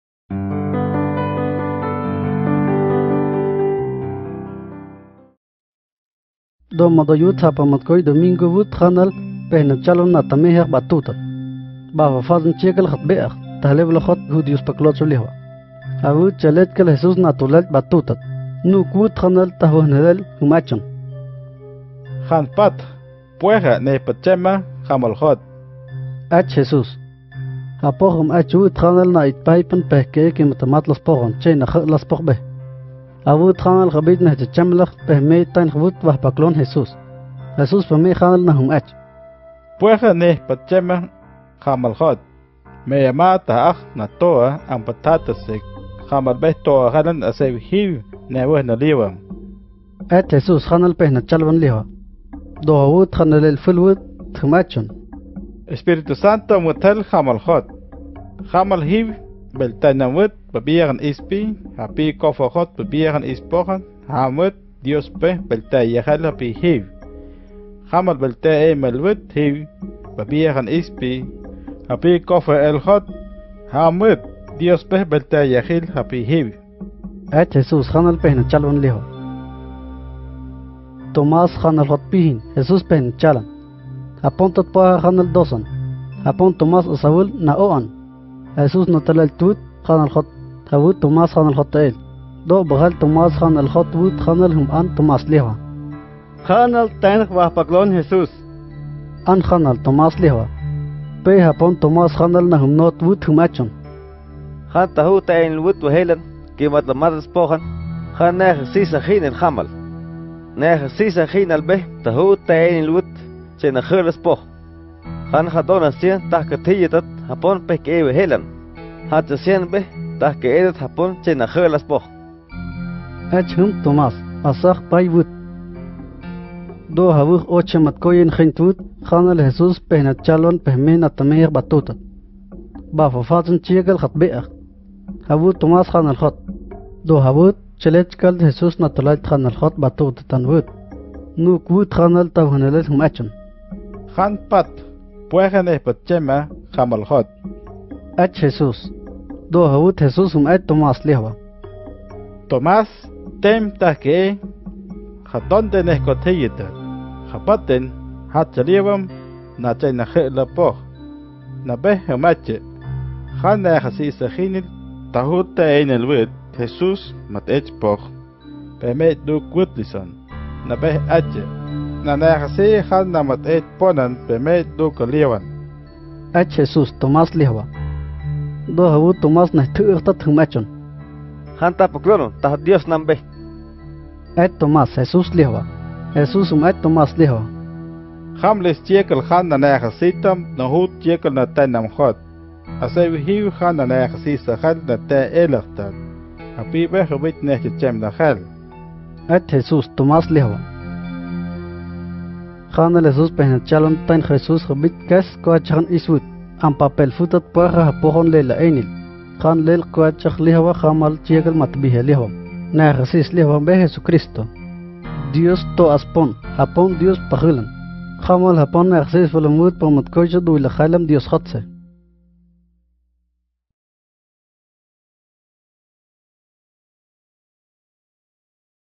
Guayabero - John 20:19-31 - Jesus appears to the disciples [guo]
Scripture Videos from the Gospel of John in Guayabero of Colombia. [guo]